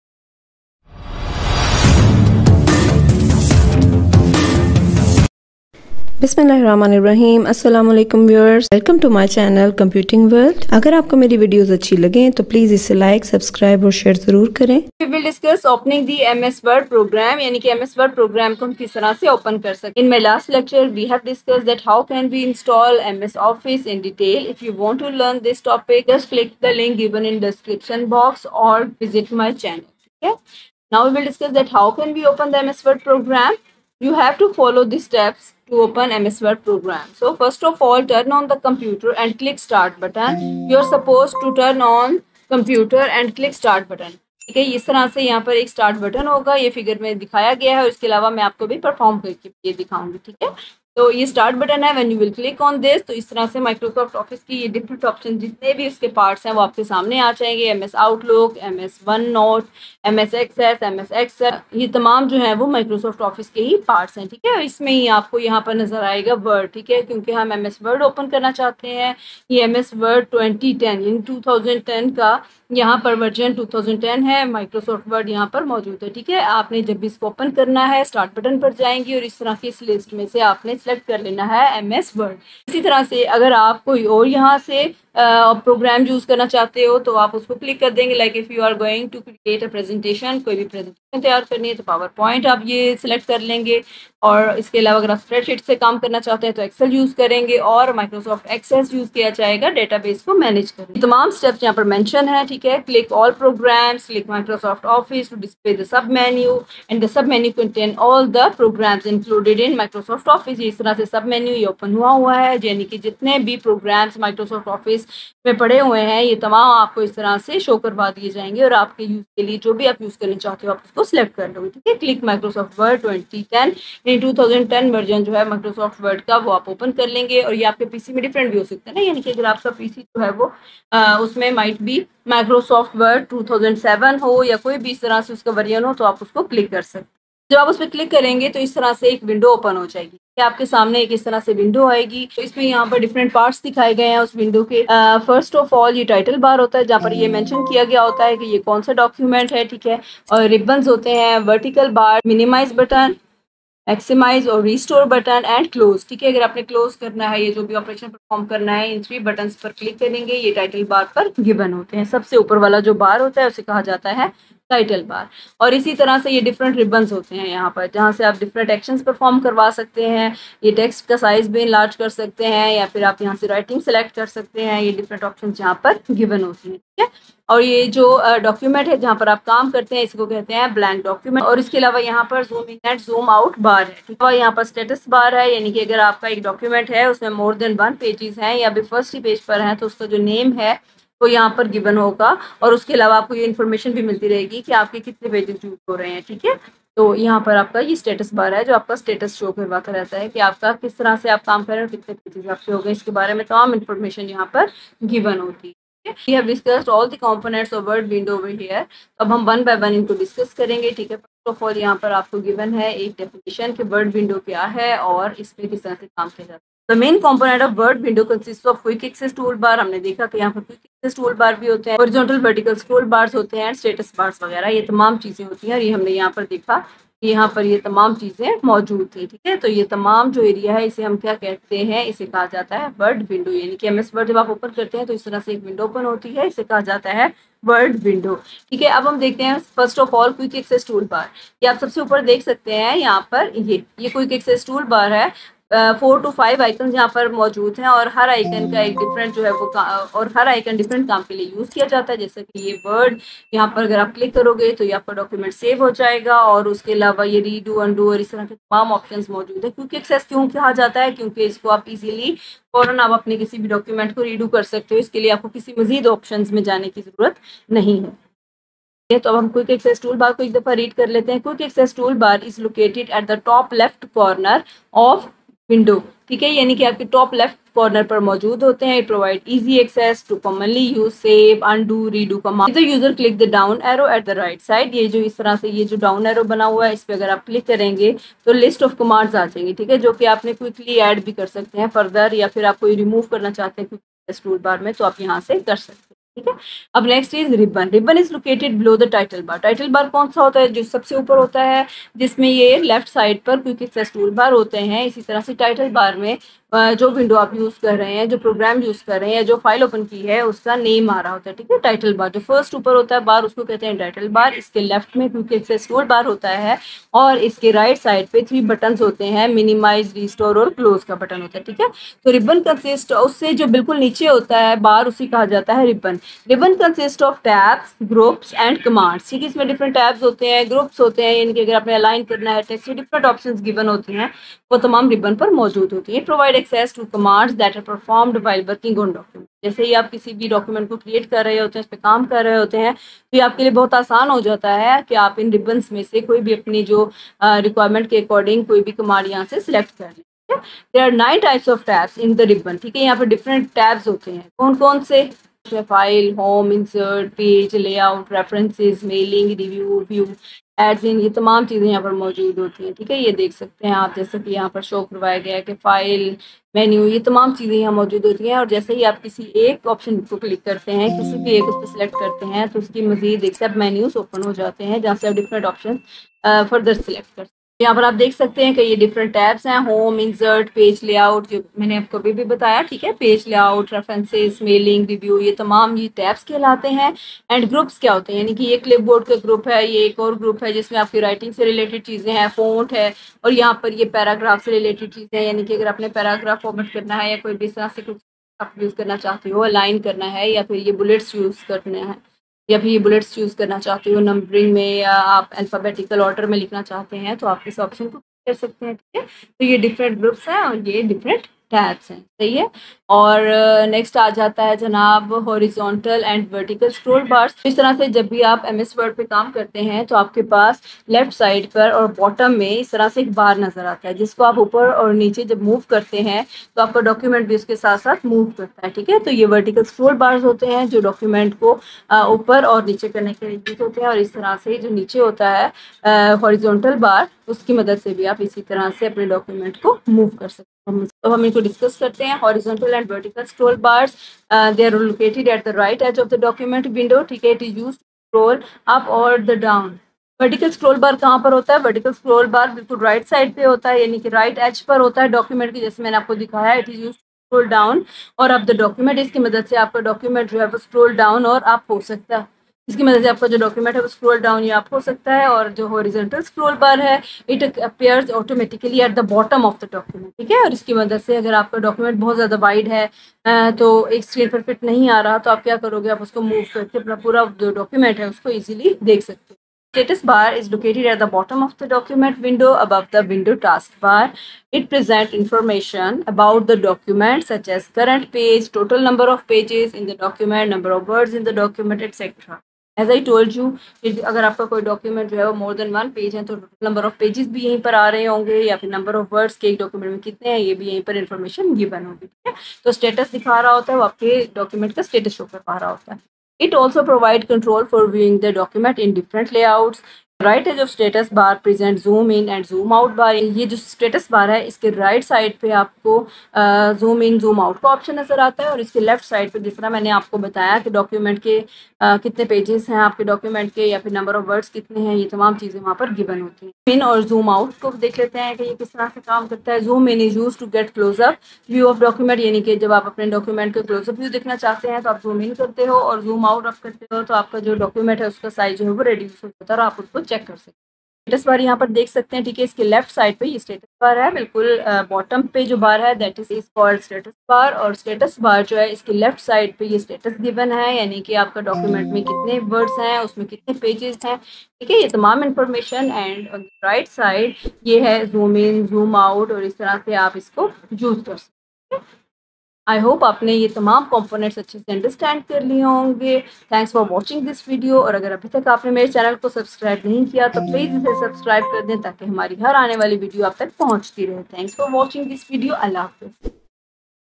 Opening the MS Word Program||Main Components of Word window||Easy Computer@ Computing World.
Assalamualaikum Everyone,

In this video we'll discuss Chapter #3: Office Automation
 1.The steps for
opening the MS Word program.

2.The main components of Word window consist of Quick Access Toolbar ,Ribbon, Horizontal/Vertical, Scroll bars and Status bar,

This video is helpful for students of class 8th, 9th and 10th, First year Computer science as well.

fundamentals of computer.

for more videos come to my channel
Channel Name:  Computing World.
Thanks.